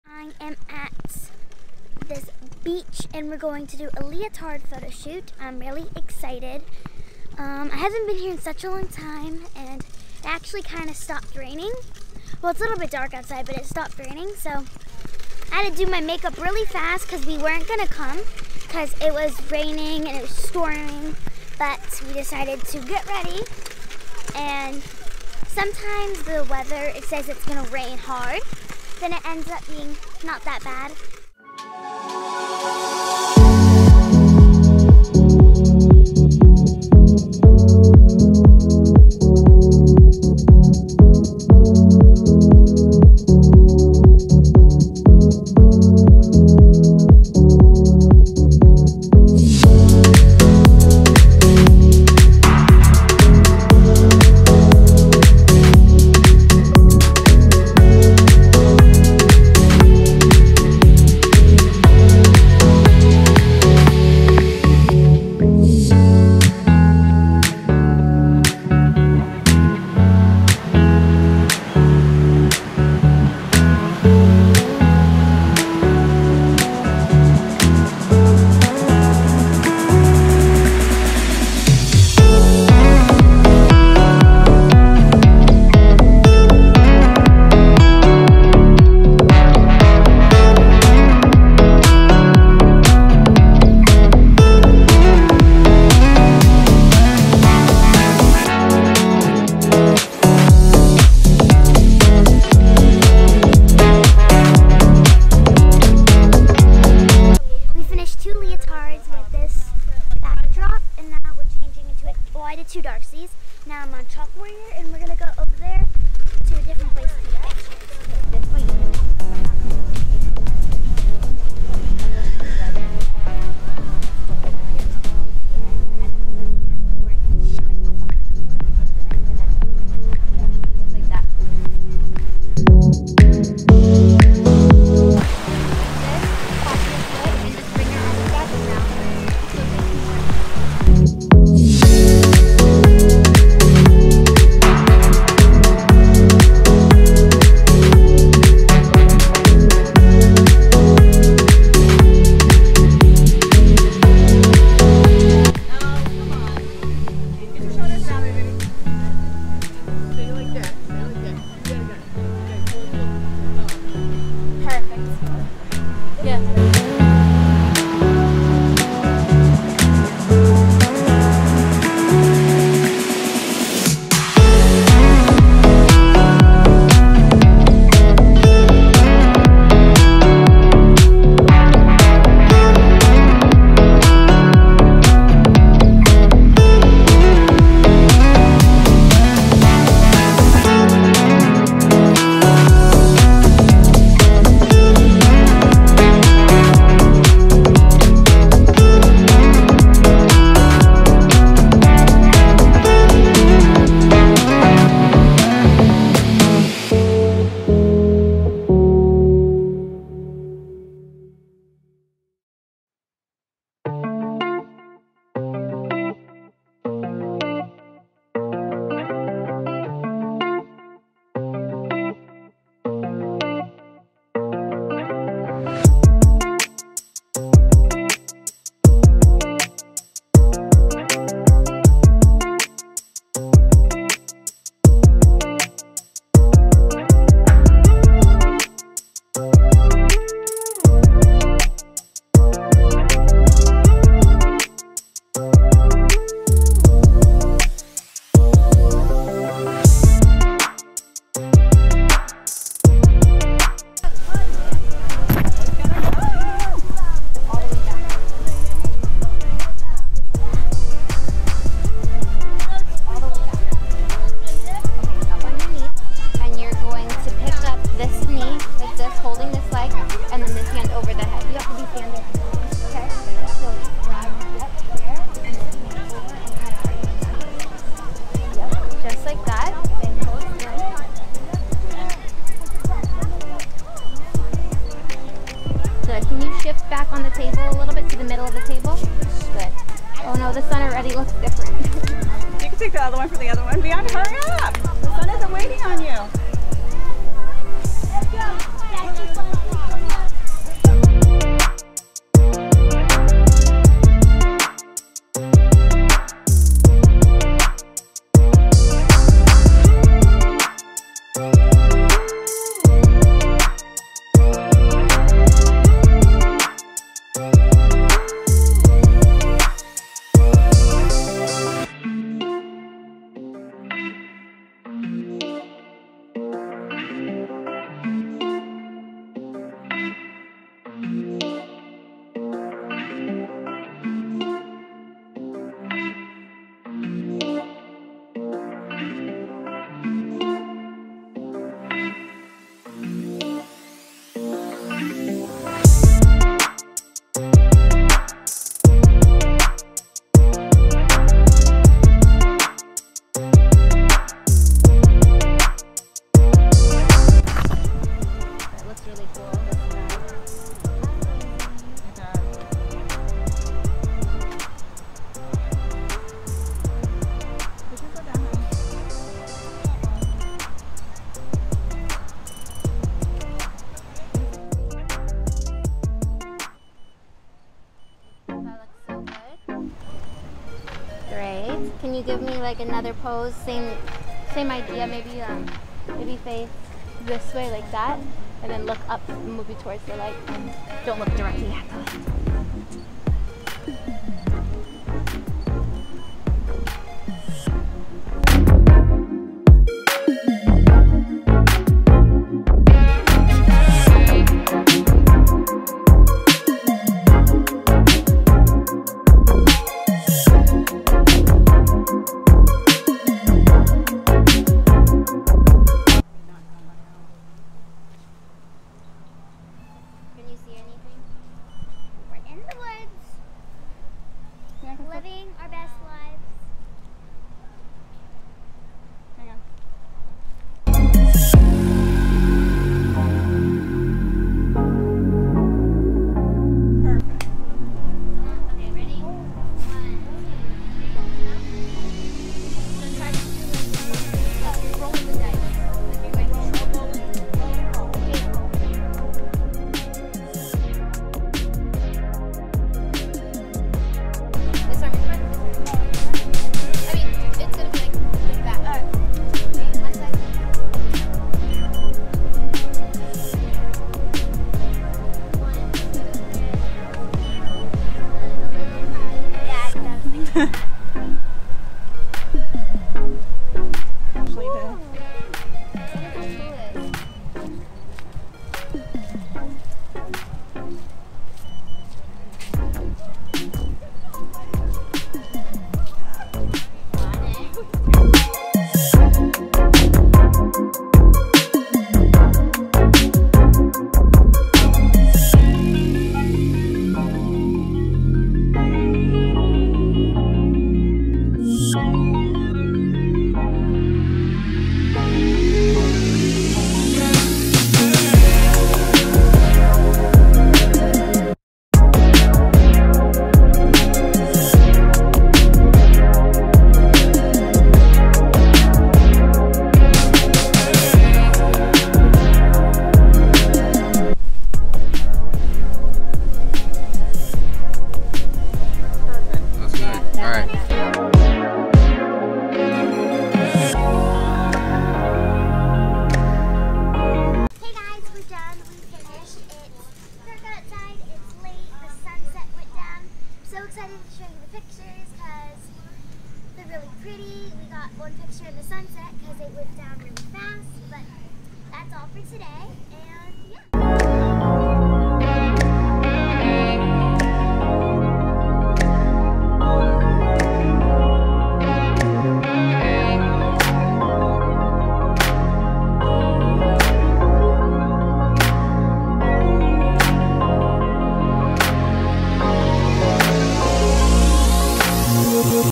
How to Rock a Leotard Photoshoot!
The cloudy skies didn’t stop Bianca from totally killing this leotard Photoshoot. With barely any rain and a gorgeous sunset, this Photoshoot turned out better than we thought.